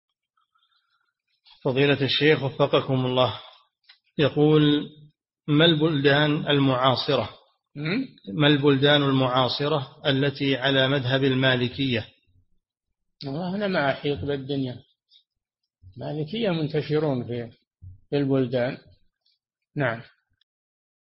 [4 -308] ما البلدان المعاصرة التي على مذهب المالكية؟ - الشيخ صالح الفوزان
-[4 -308] السؤال: ما البلدان المعاصرة التي على مذهب المالكية؟
الجواب: والله أنا ما أحيط بالدنيا المالكية منتشرون في البُلدان.
للاشتراك في قناة الشيخ صالح الفوزان

للاستفسار والإقتراحات والدعم
رئيس الفريق
لمشروع كبار العلماء